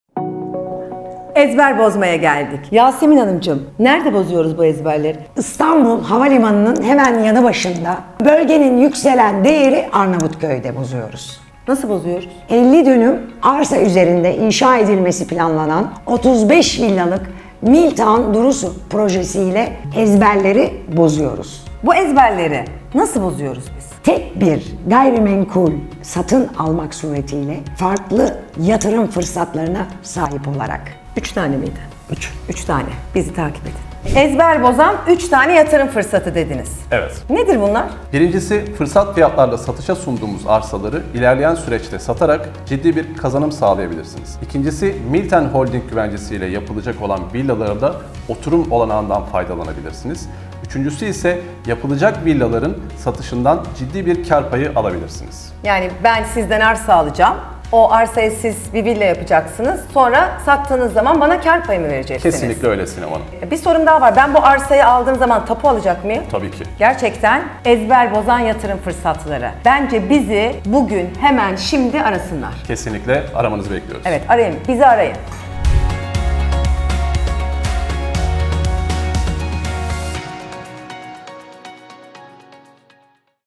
ARNAVUTKÖY BOYALIK Miltown Durusu U SATILIK ARSA (KÂRLI YATIRIM FIRSATI)
Miltown Durusu Projesi: Arnavutköy’de Kârlı Gayrimenkul Yatırım Fırsatı
Arsanıza Bugün Sahip Olun, Geleceğinize Yatırım Yapın!

Gayrimenkul yatırımında devrim yaratan Miltown Durusu Projesi ile tanışın! 53 dönümlük geniş bir alanda inşa edilecek bu projede, size özel arsa yatırım seçenekleri sunuyoruz. Tek bir arsa üzerinde, bütçenize uygun ve yüksek kazanç potansiyeline sahip farklı yatırım alternatifleriyle finansal geleceğinizi güvence altına alın.

Sizi Kâr Ettirecek Yatırım Seçenekleri:

1. Fırsat Fiyatla Arsa Satın Alımı: Şimdiden bir dönüm arsa alarak yatırım yapın ve ilerleyen süreçte bu arsayı satarak ciddi kazançlar elde edin. Arnavutköy Boyalık mevkiinde arsa fiyatları hızla yükseliyor, bu yüzden fırsatı kaçırmayın!

2. Kâr Payı ile Villa Satışı: Arsanız üzerine inşa edilecek olan lüks villaların satışı sonrası, satış bedeli üzerinden kâr payınızı alarak büyük bir getiri elde edin. Milten Holding güvencesi ile villalar, prestijli bir yaşam ve yatırım fırsatı sunuyor.

3. Lüks Villalarda Yaşam: Milten Holding’in yüksek kalitesi ile inşa edilecek olan lüks villalarda yaşama olanağına sahip olun. Miltown Durusu’nun sunduğu ayrıcalıklarla İstanbul’un hızla yükselen değeri Arnavutköy’de muhteşem bir gayrimenkul yatırımı yapmış olacaksınız.

Arnavutköy Boyalık Mevki: Yatırım İçin Mükemmel Lokasyon

Miltown Durusu Projesi, İstanbul’un hızla gelişen ve değer kazanan bölgelerinden biri olan Arnavutköy Boyalık mevkiinde yer almaktadır. Bu bölge, gelecekteki mega projelere olan yakınlığı ve doğayla iç içe konumu ile dikkat çekmektedir. Yeni İstanbul Havalimanı’na yakınlığı, ulaşım ağlarına kolay erişim sağlaması ve İstanbul Kanalı gibi büyük projelere olan yakınlığı ile Arnavutköy, gayrimenkul yatırımı için ideal bir bölgedir.

Arnavutköy Boyalık Mevkii'nin Özellikleri:

Stratejik Konum: İstanbul’un yeni gelişim alanlarına yakın, hızlı erişim imkânı.

Yüksek Kazanç Potansiyeli: Bölgedeki gayrimenkul değer artışı sayesinde yüksek yatırım getirisi.

Doğayla İç İçe Yaşam: Yeşil alanlar ve doğal güzellikler ile çevrili huzurlu bir yaşam alanı.

Miltown Durusu Projesi ile Güvenli ve Kârlı Yatırım

Milten Holding’in güvencesi altında, Miltown Durusu Projesi ile hem yüksek kaliteli yaşam alanlarına sahip olacak, hem de değerine değer katacak bir yatırım yapacaksınız. Bu fırsatı kaçırmayın; hemen bize ulaşarak detaylı bilgi alın ve geleceğinizi şekillendirin!

Detaylı bilgi ve arsa seçenekleri için hemen bizi arayın